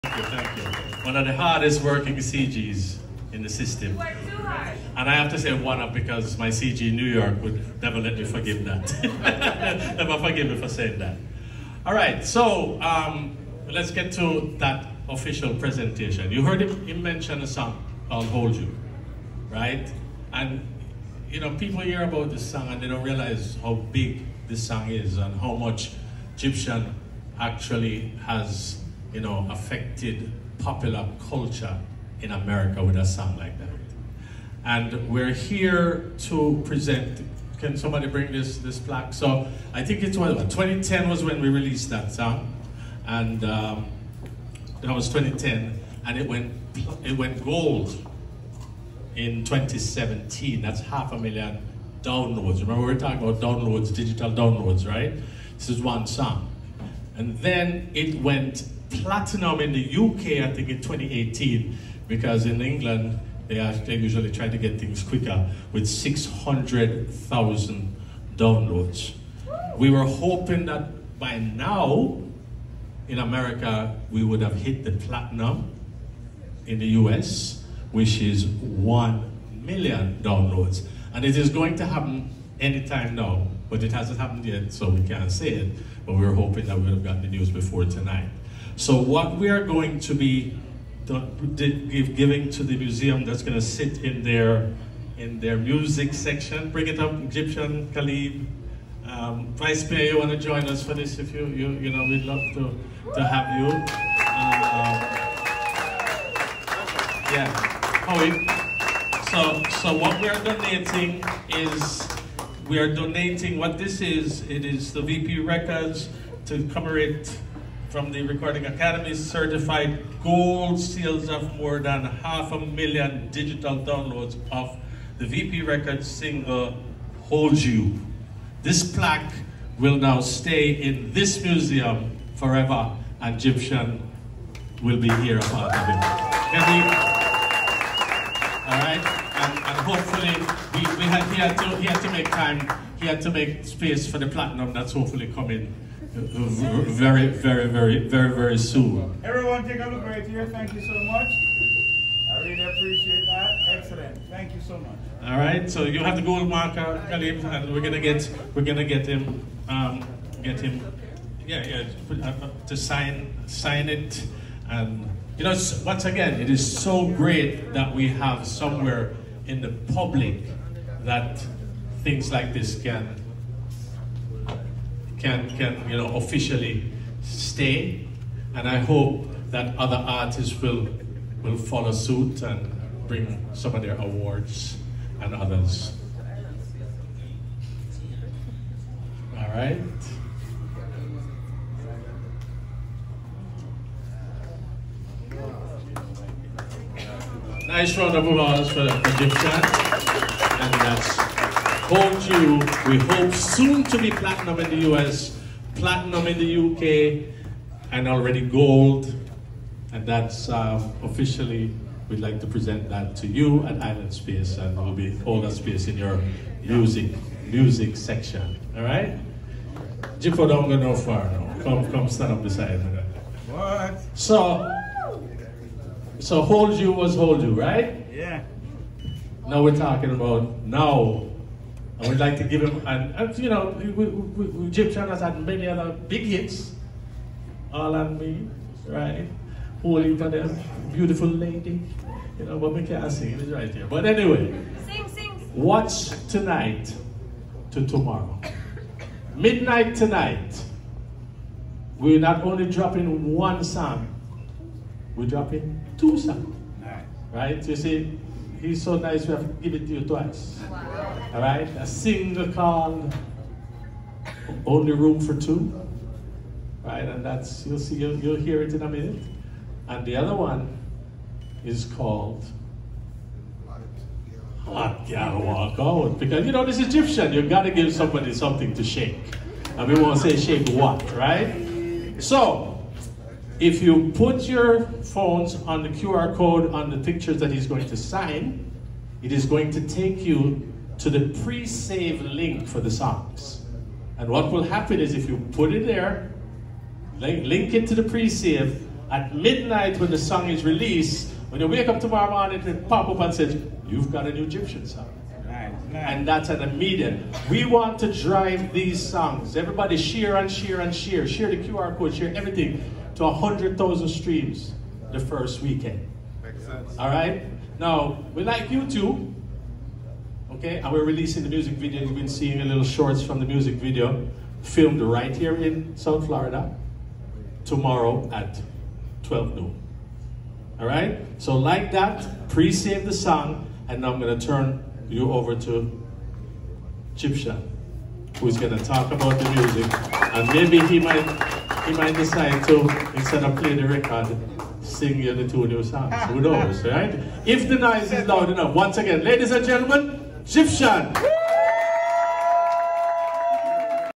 Gyptian Makes Presentation to Island SPACE Caribbean Museum
VP Records artist Gyptian makes presentation to Island SPACE Caribbean Museum at Broward Mall in Plantation.